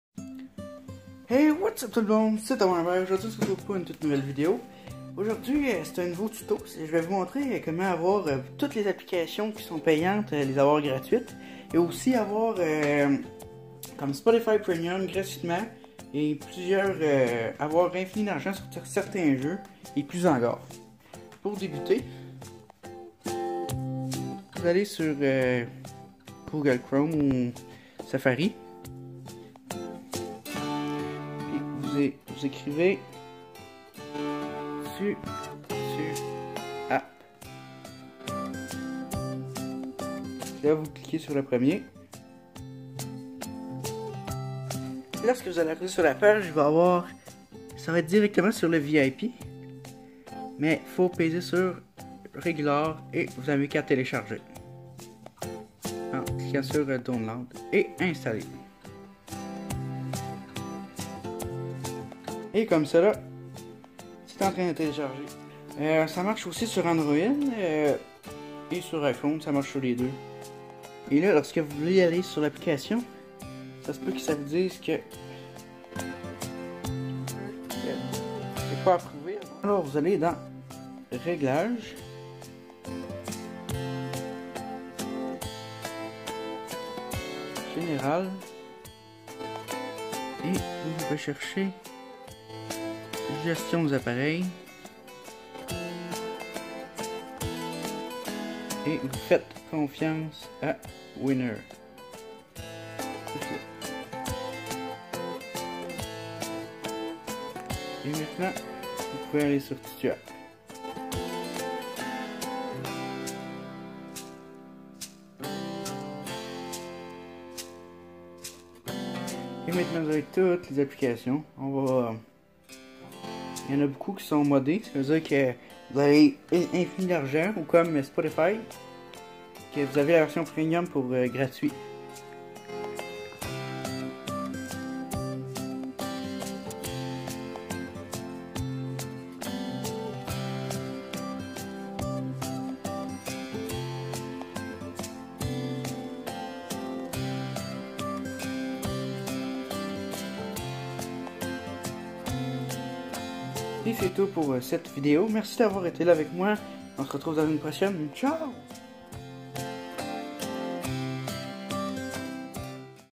Tuto Tutuapp comment avoir des apps gratuite
Voici un tuto pour télécharger Tutuapp
vous pouvez le téléchargé sur android et ios

P.s Tutuapp marche mieux sur Android
Sa se peut que vous avez de la misère à télécharger spotify++ sur iOS